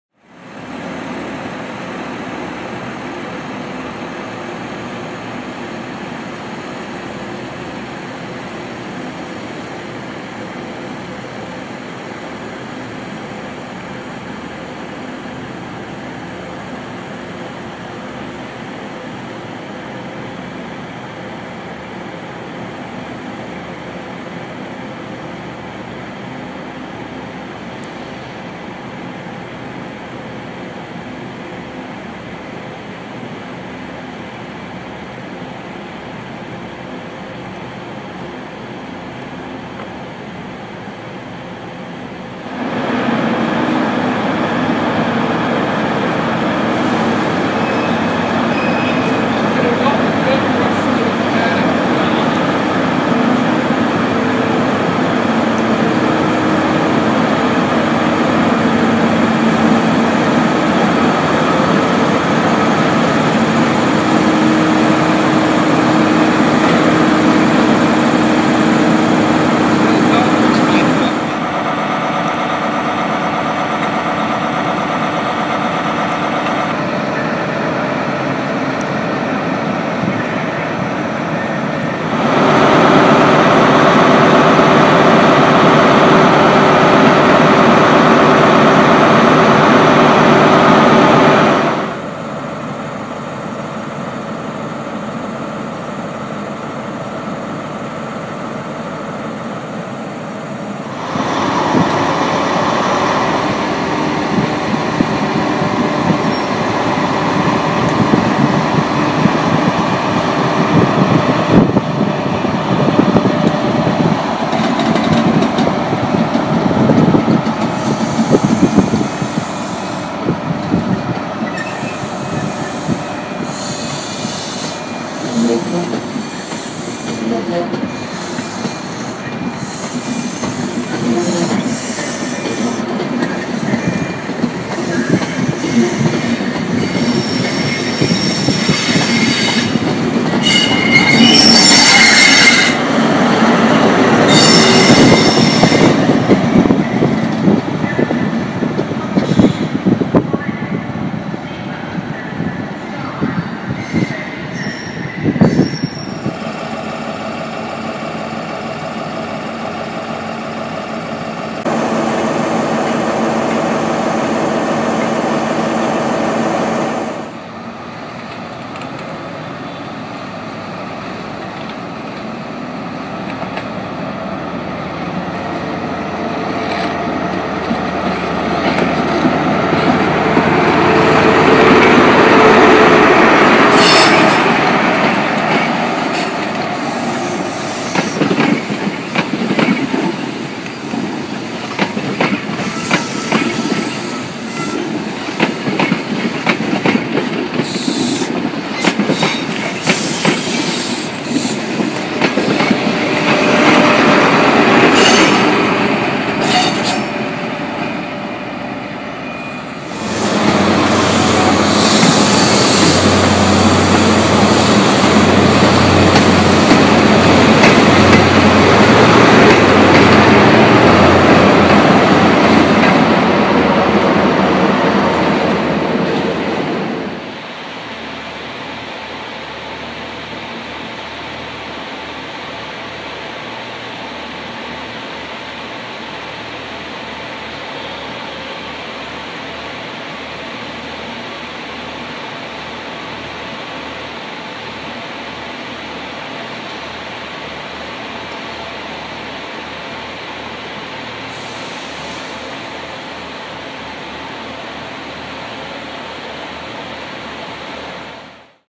Class 43s
All the times I spotted a Hull Trains Class 43 in Hull Paragon Interchange.